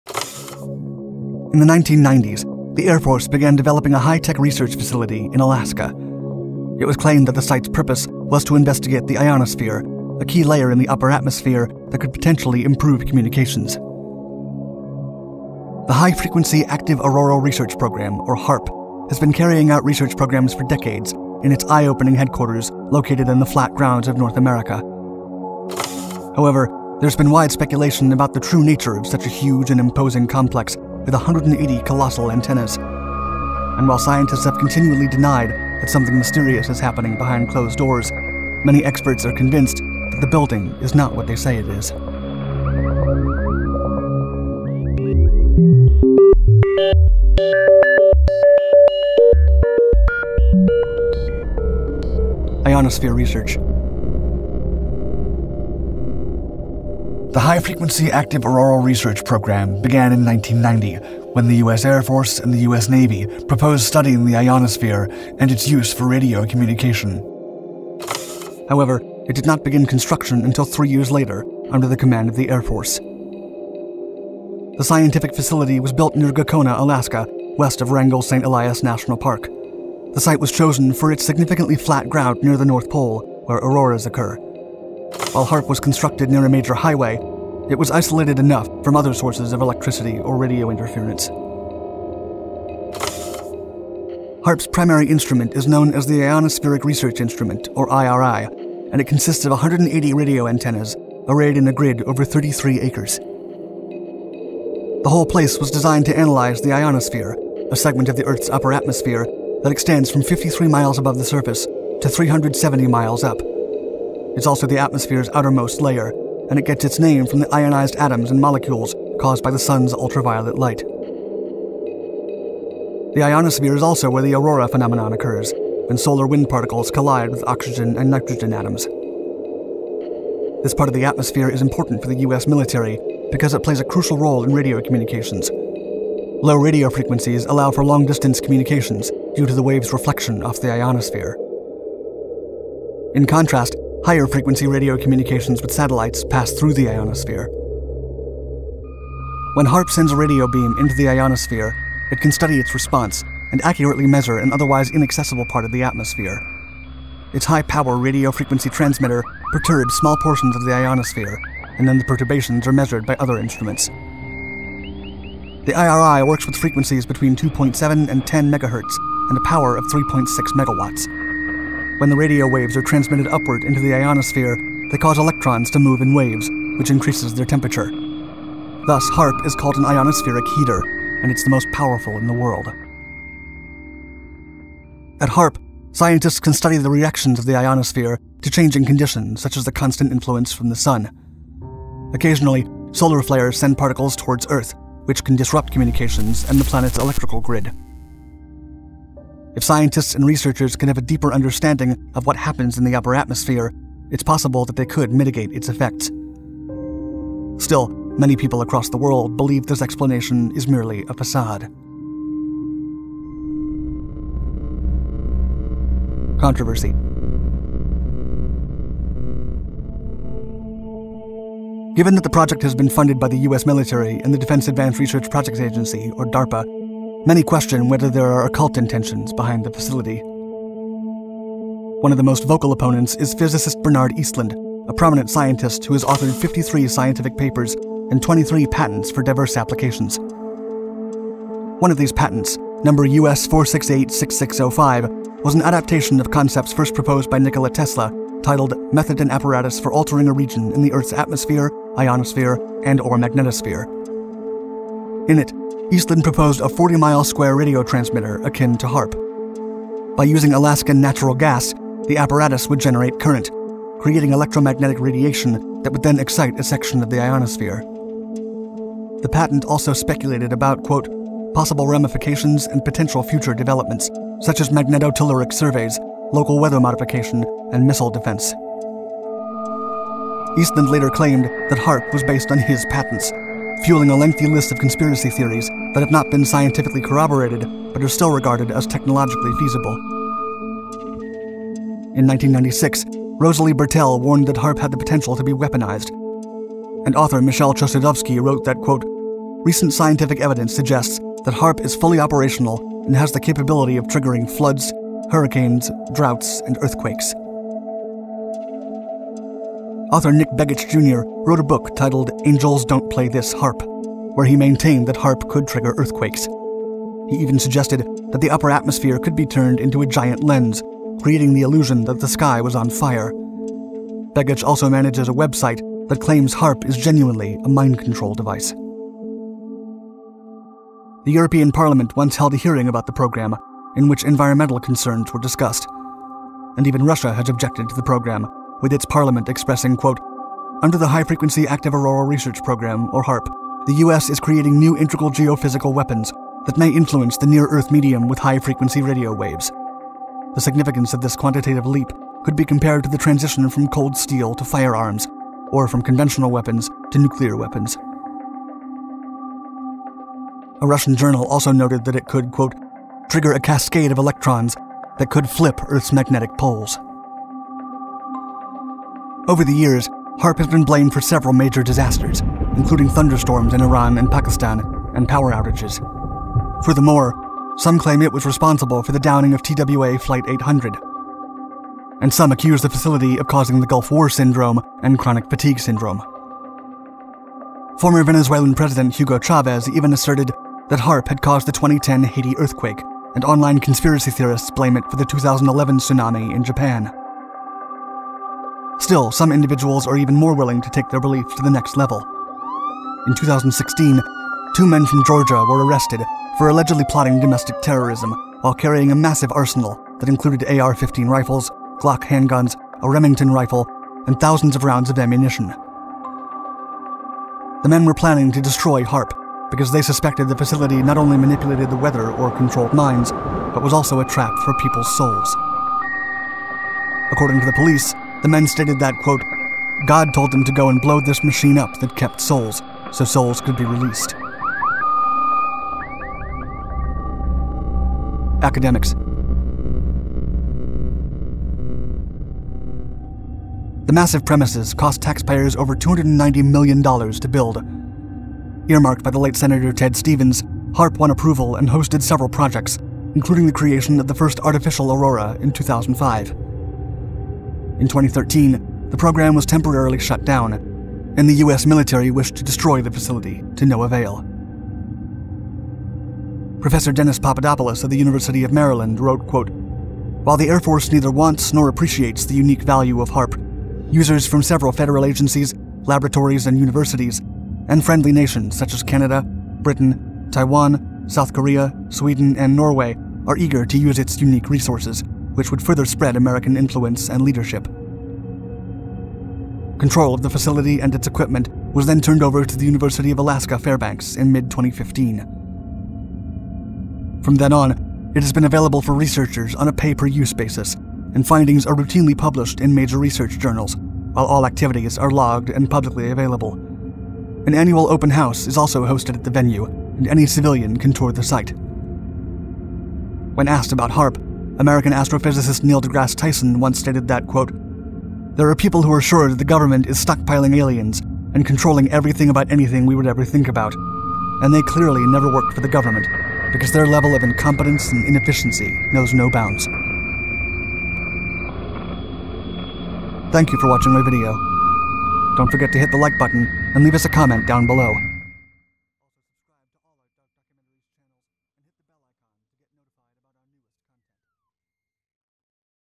Earth's Strangest Experiment: The Mysterious Project HAARP
In the 1990s, the Air Force began developing a high-tech research facility in Alaska. It was claimed that the site’s purpose was to investigate the ionosphere, a key layer in the upper atmosphere that could potentially improve communications.

The High-frequency Active Auroral Research Program, or HAARP, has been carrying out research programs for decades in its eye-opening headquarters located in the flat grounds of North America.

However, there has been wide speculation about the true nature of such a huge and imposing complex with 180 colossal antennas. And while scientists have continually denied that something mysterious is happening behind closed doors, many experts are convinced that the building is not what they say it is...